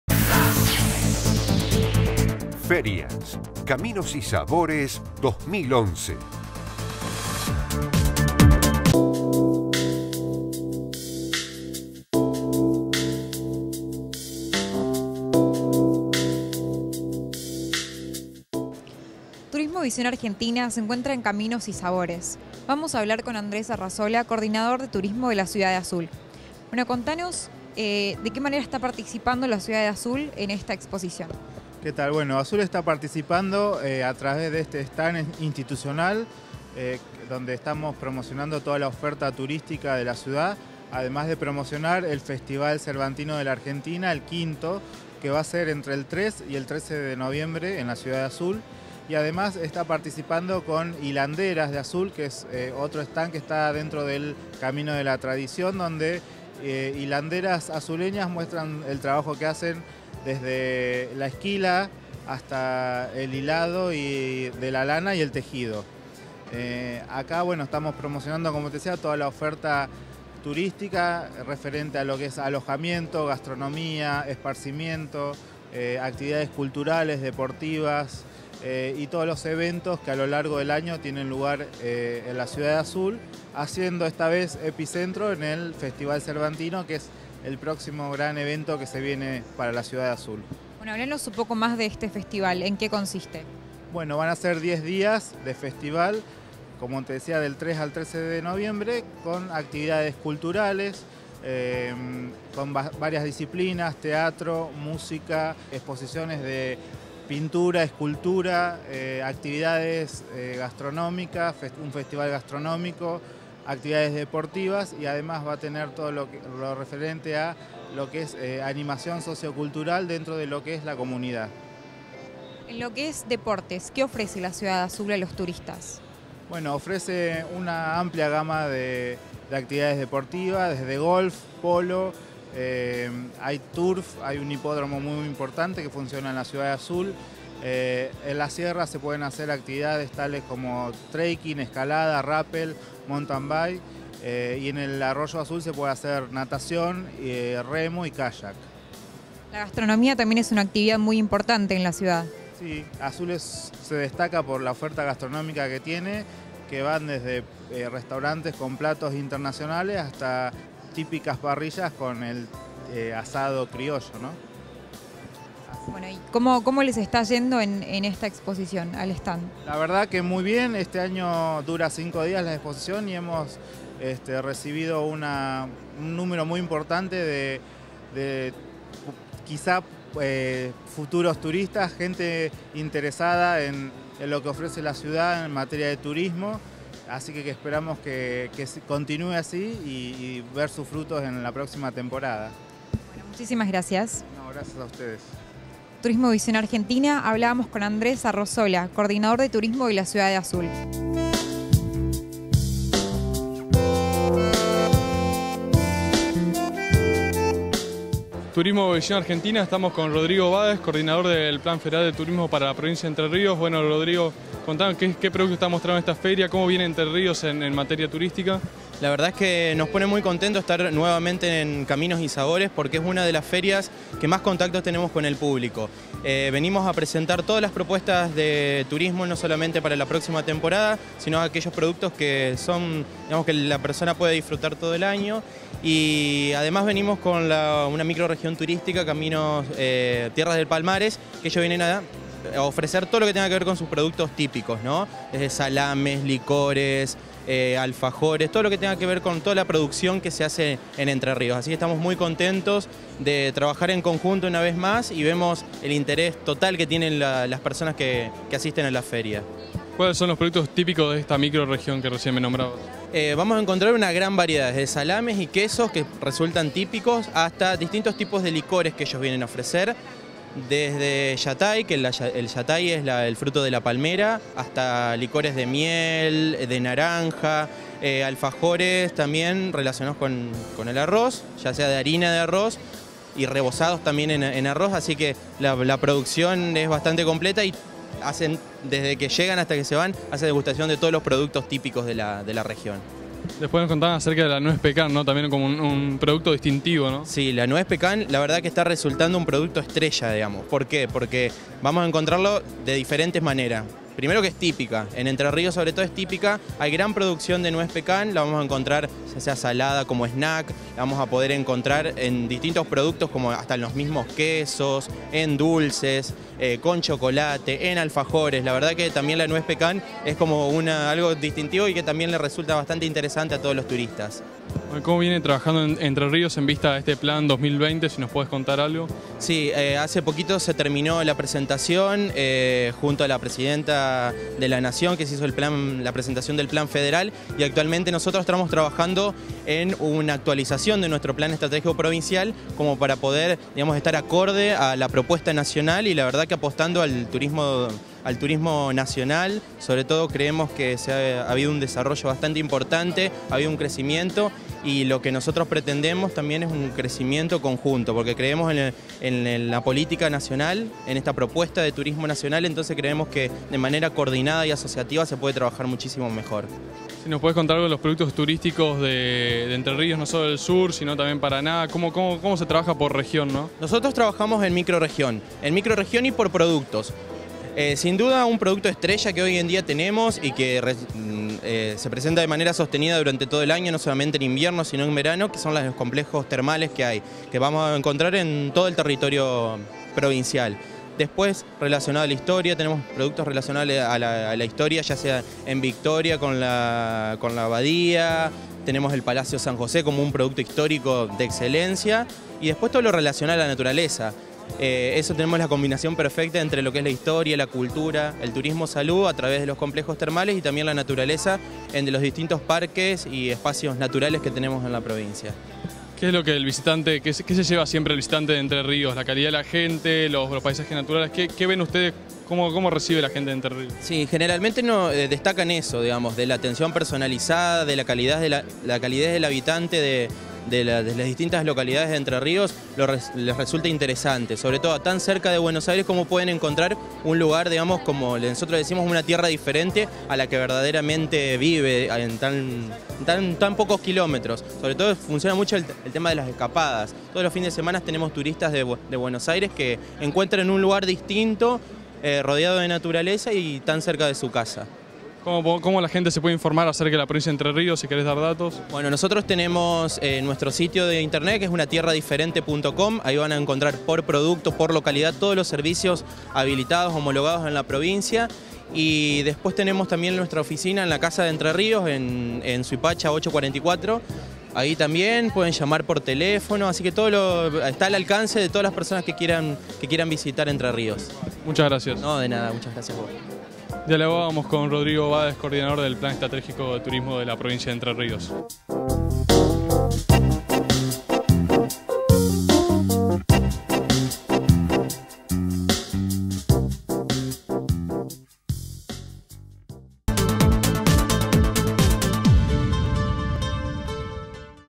Ferias: Caminos y Sabores 2011
Turismo Visión Argentina presente en en una nueva edición de Caminos y Sabores. Allí hablamos con Andrés Arrazola, Coordinador de Turismo de Azul. También dialogamos con Rodrigo Báez, Coordinador del PEDTS, en Entre Ríos.